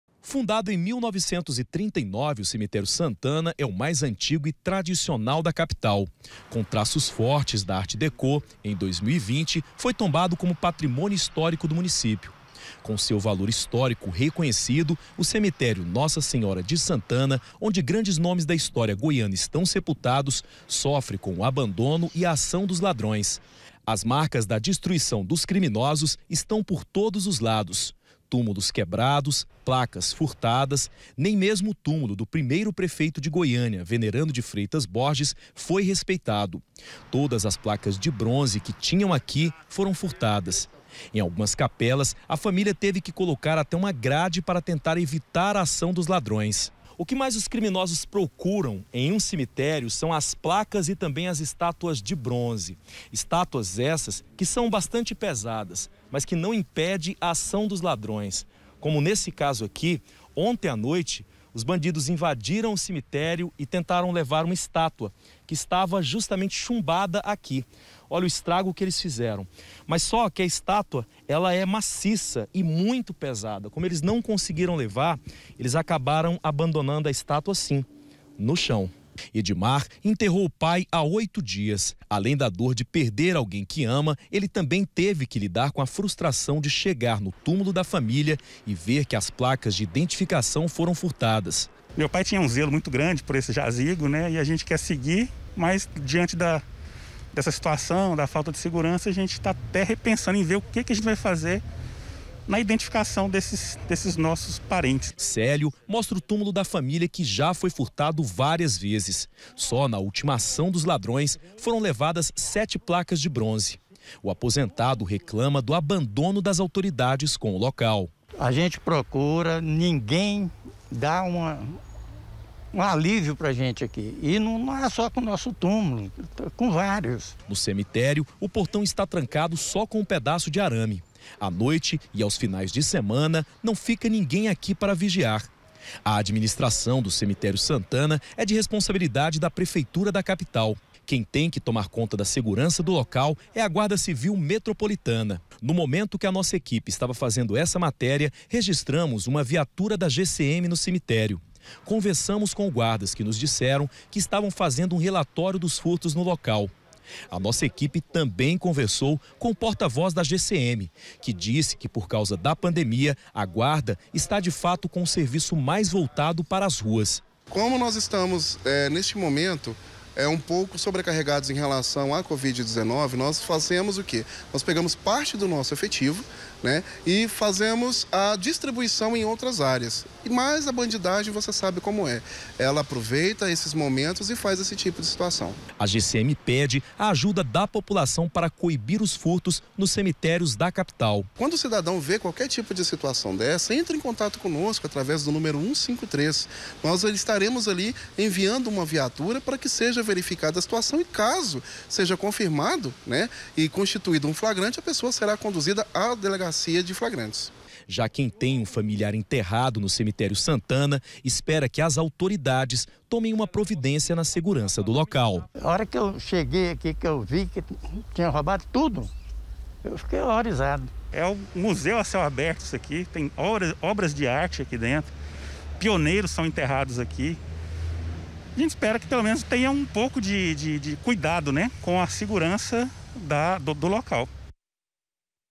PATRIMÔNIO AMEAÇADO: VÂNDALOS DESTROEM TÚMULOS E FURTAM OBJETOS
O CEMITÉRIO SANTANA, TOMBADO COMO PATRIMÔNIO HISTÓRICO DE GOIÂNIA, É ALVO DA AÇÃO DE VÂNDALOS. OS CRIMINOSOS ESTÃO DESTRUINDO TÚMULOS E FURTANDO PRA DEPOIS VENDER.